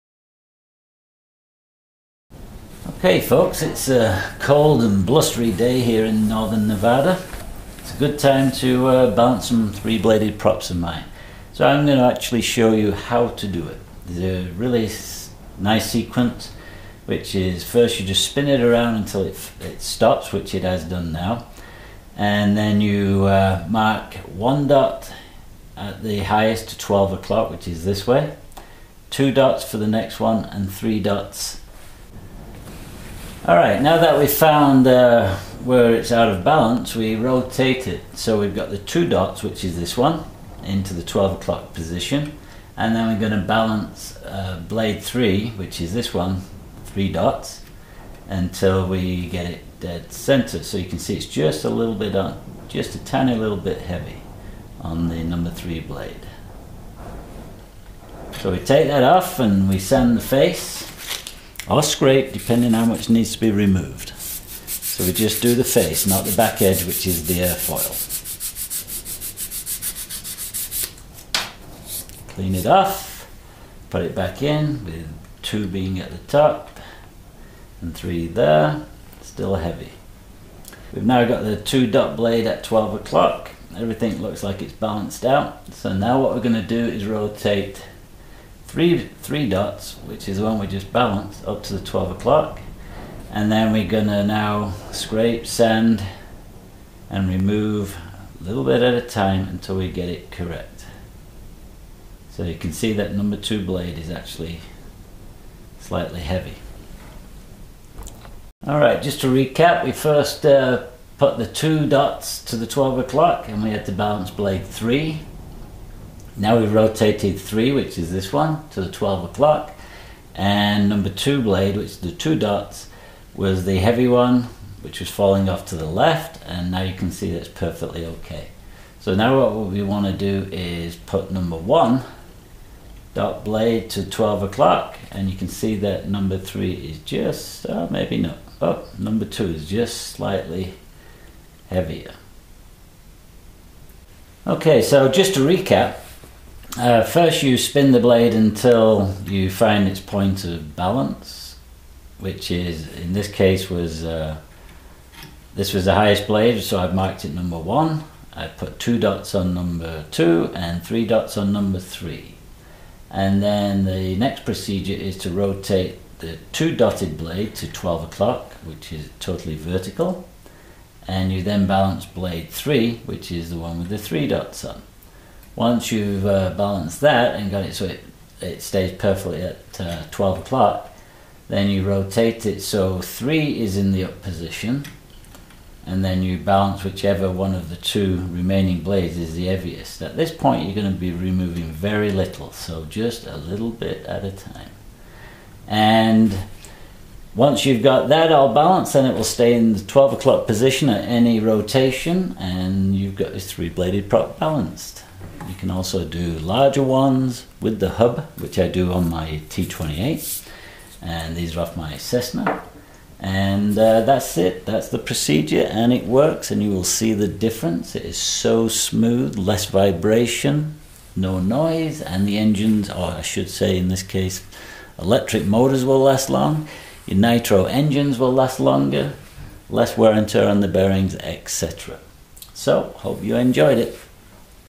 3 Bladed Prop Balancing for RC Aircraft
How to correctly balance 3 blade Propeller for Radio Control Model Aircraft